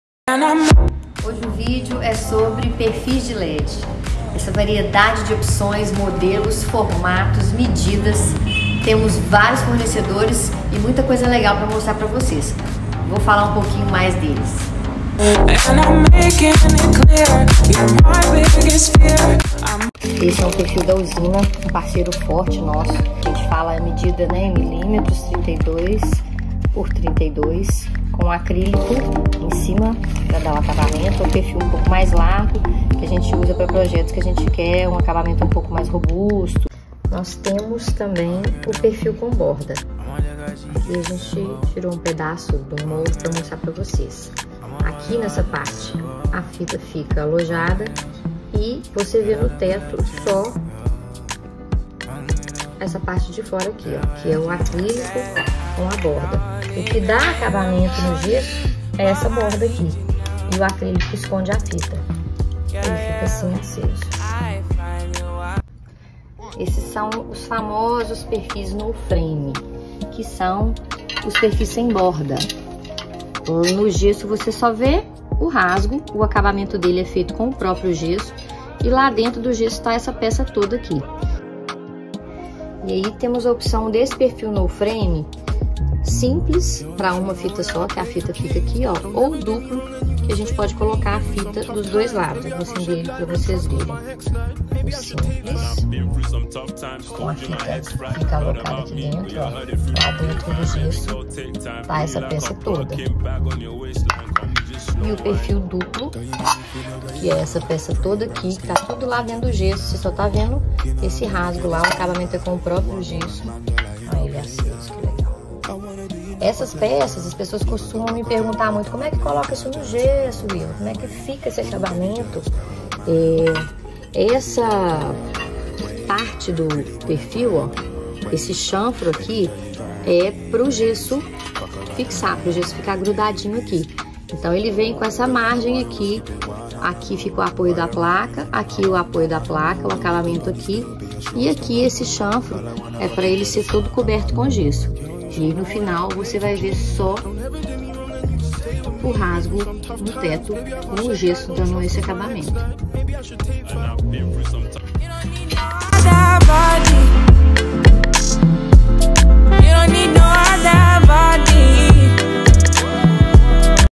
TUDO SOBRE PERFIS DE LED - SETEMBRO/2022
Quer saber mais sobre como usar Perfis em seu projeto? Aqui mostramos vários modelos sem complicação!